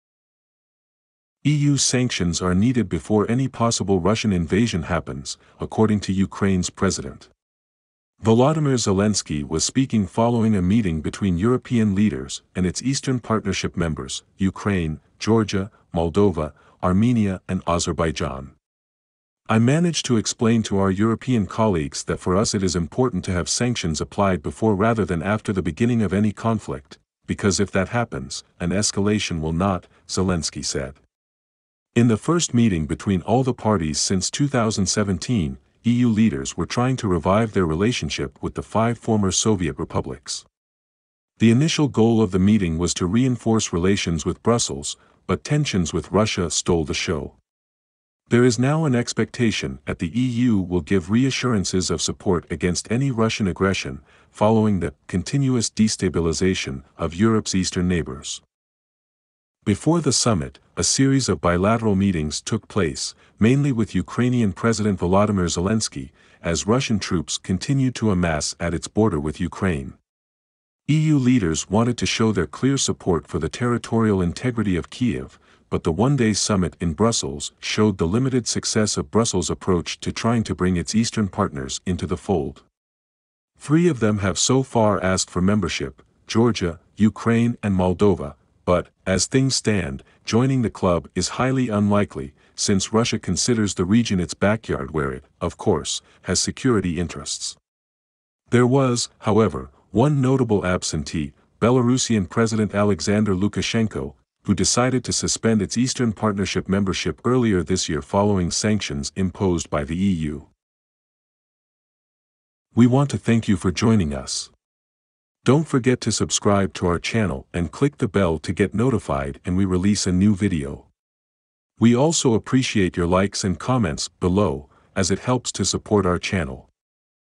Sanctions needed before any Russian invasion, says Ukrainian president!
EU sanctions are needed before any possible Russian invasion happens, according to Ukraine's president.

Volodymyr Zelenskyy was speaking following a meeting between European leaders and its Eastern Partnership members - Ukraine, Georgia, Moldova, Armenia and Azerbaijan.

"I managed to explain to our European colleagues that for us it is important to have sanctions applied before rather than after the beginning of any conflict...because if that happens, an escalation will not," Zelensky said.

In the first meeting between all the parties since 2017, EU leaders were trying to revive their relationship with the five former Soviet republics.

The initial goal of the meeting was to reinforce relations with Brussels, but tensions with Russia stole the show.

There is now an expectation that the EU will give reassurances of support against any Russian aggression, following the "continuous destabilisation" of Europe’s eastern neighbours.

Before the summit, a series of bilateral meetings took place, mainly with Ukrainian President Volodymyr Zelenskyy, as Russian troops continued to amass at its border with Ukraine.

EU leaders wanted to show their clear support for the territorial integrity of Kyiv, but the one-day summit in Brussels showed the limited success of Brussels' approach to trying to bring its eastern partners into the fold.

Three of them have so far asked for membership: Georgia, Ukraine and Moldova, but, as things stand, joining the club is highly unlikely, since Russia considers the region its backyard where it, of course, has security interests.

There was, however, one notable absentee, Belarusian President Alexander Lukashenko, who decided to suspend its Eastern Partnership membership earlier this year following sanctions imposed by the EU.

IMPORTANT!
-None of these images, music & video clips were created/owned by us. We will respectfully remove it immediately.